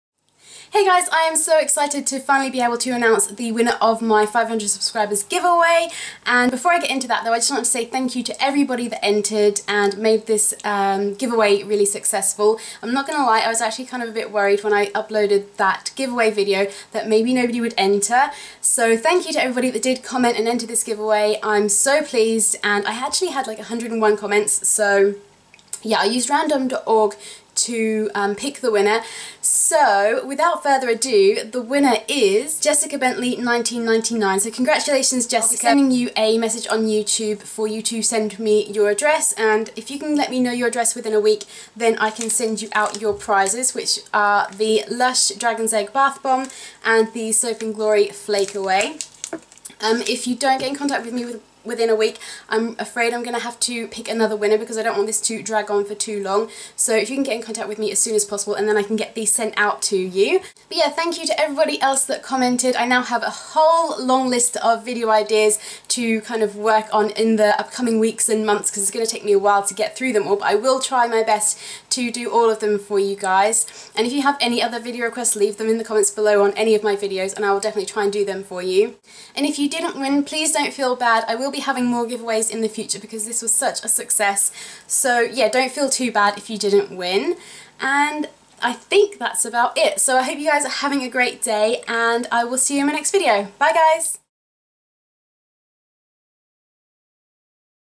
500 Subscribers Giveaway Winner!
Links:
My life in pictures..Instagram!!

And the winner is. JessicaBentley1999!